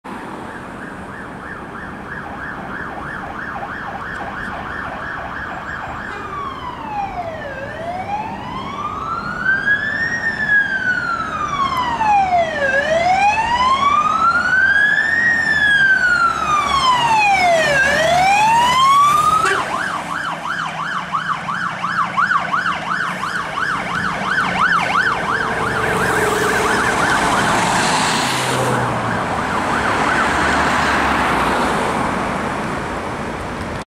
Merseyside Fire & Rescue Service Kirkdale CPL Responding
*Rare* Merseyside Fire & Rescue Service Kirkdale CPL M10A1, Responding along Leeds Street in Liverpool on blue lights and wailers to unknown call this evening (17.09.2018). Kirkdale Pump had gone passed first and was filmed in another video.